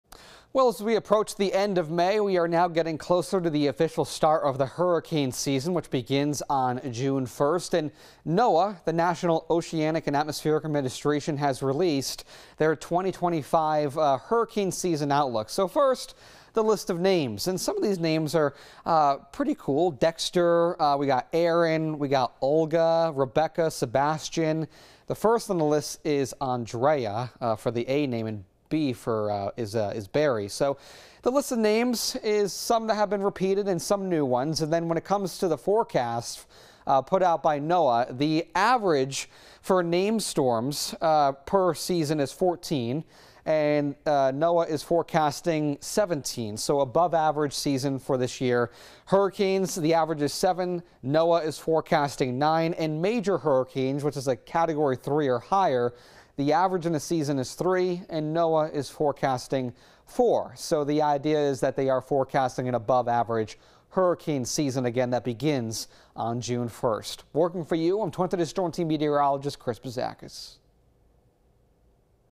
A look at the new NOAA hurricane forecast
22News Storm Team Meteorologist Chris Bouzakis has a look at the new hurricane season outlook from NOAA.